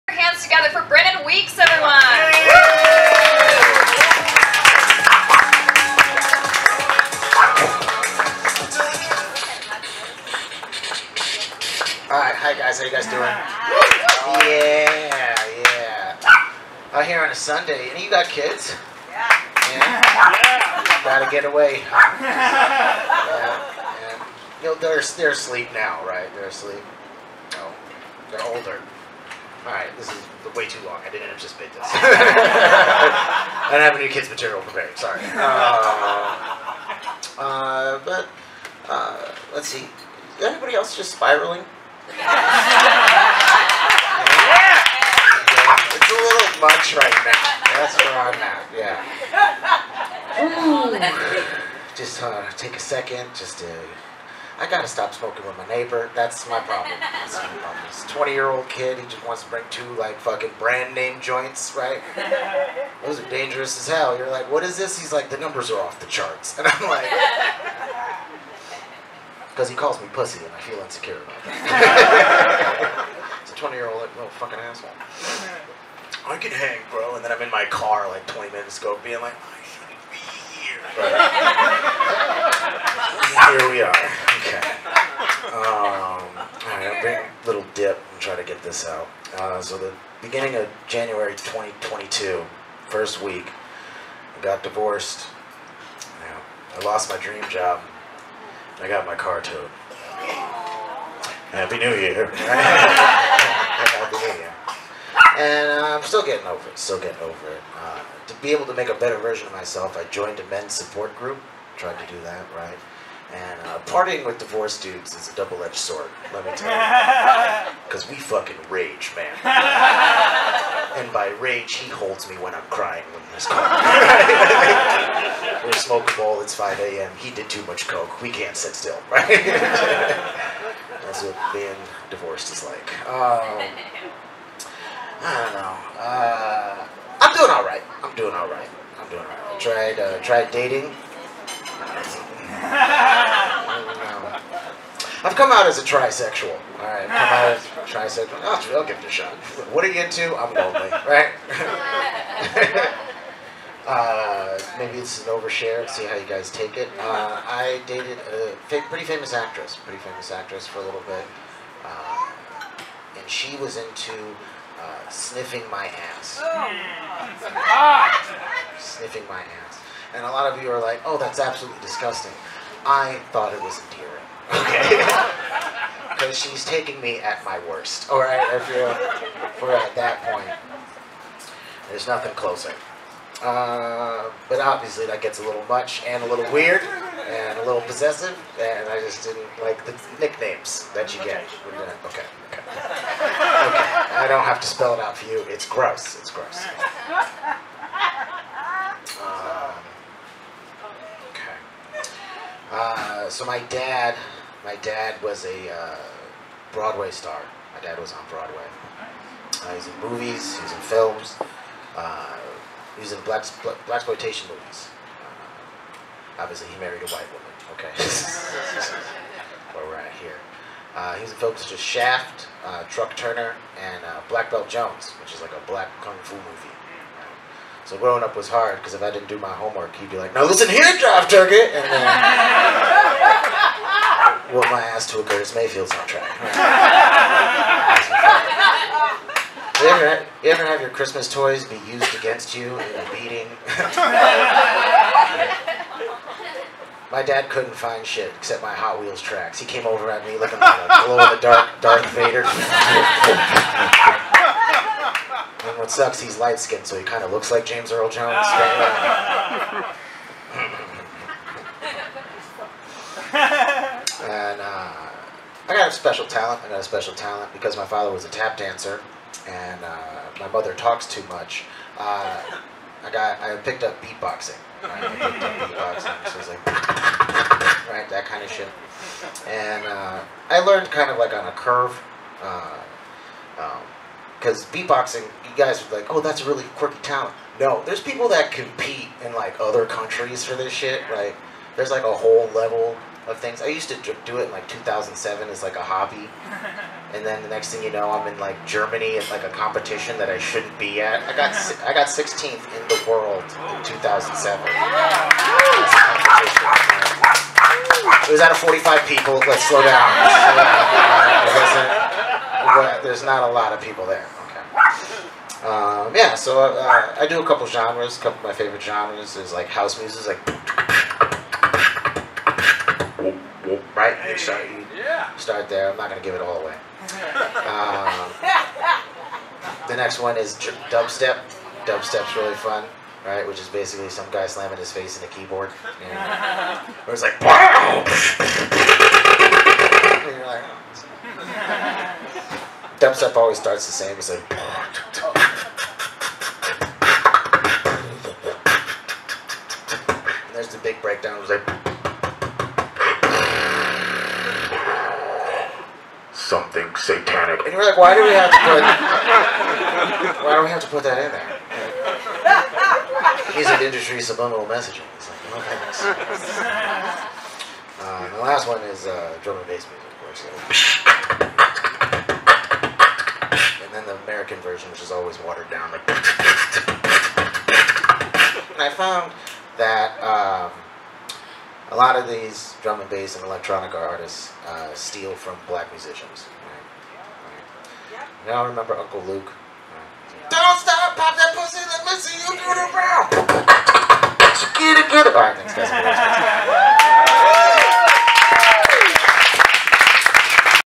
Brinan Weeks at the Dead Pan Show
At Fox and Hounds in Studio City, CA 08/1/22